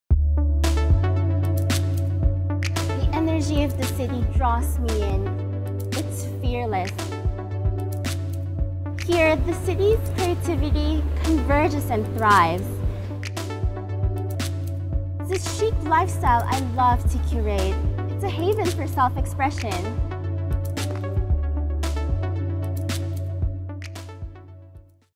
Shangri-La at the Fort, Manila - Shopping
The energy of BGC draws you in; it’s fearless! Here, the city’s creativity converges and thrives. A myriad of shopping choices are in store, reflecting a haven of self-expression and a chic lifestyle.